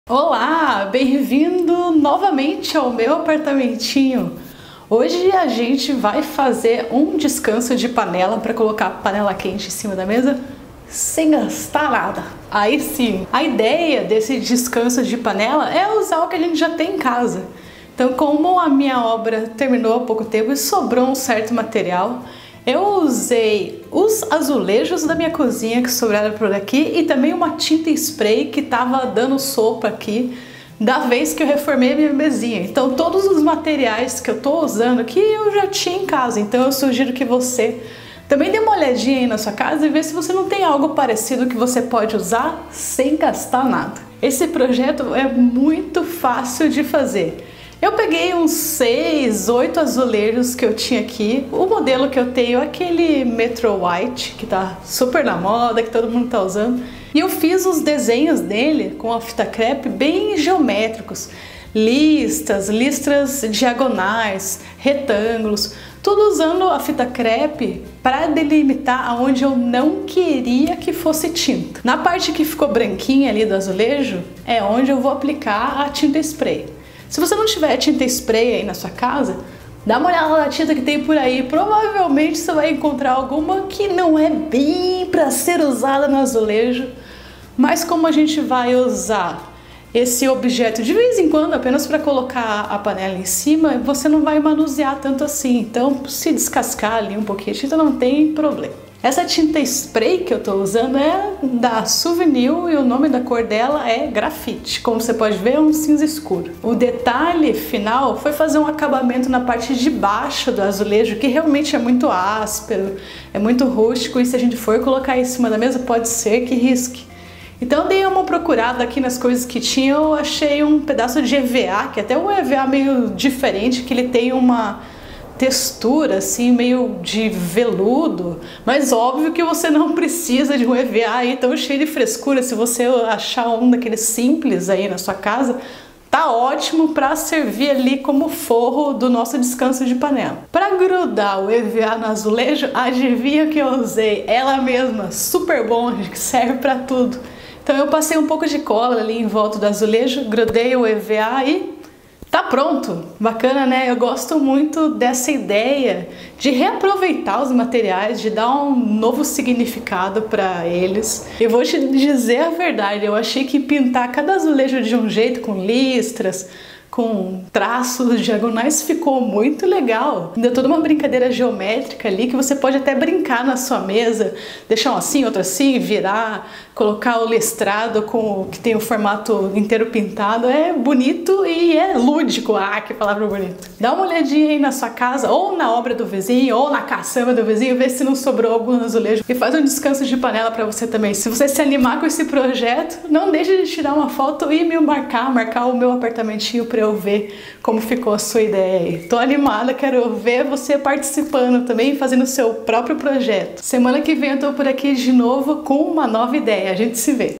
Descanso de panelas feito com azulejo
Aprenda nesse DIY como fazer um descanso para panela sem gastar nada. Eu usei os restos de azulejo da minha obra e a tinta spray da Suvinil que tinha sobrado da reforma da minha mesinha.

O azulejo é o Mind de Gap na cor Beatle da Decortiles.
Veja mais em: @MeuApartamentinho